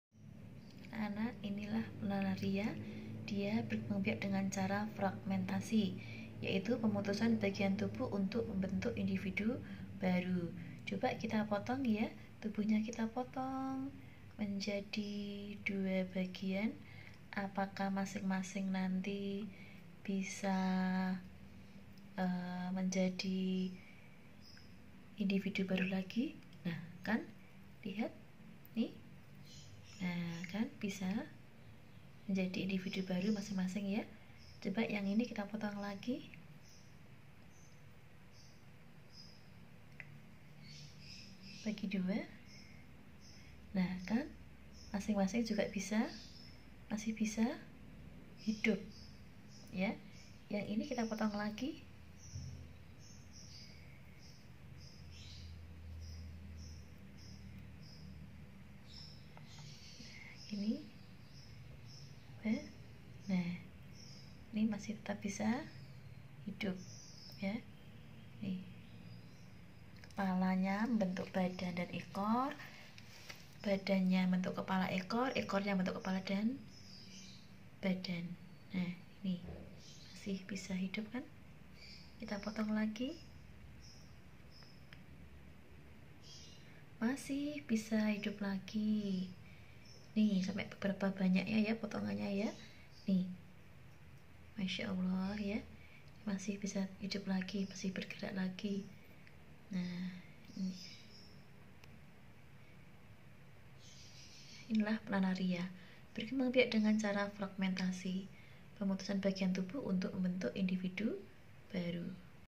Fragmentasi pada Planaria
Planaria ,salah satu cacing dari kelompok Plathyhelminthes yang berkembangbiak dengan cara Fragmentasi yaitu pemutysan bagian tubuh untuk membentuk individu baru.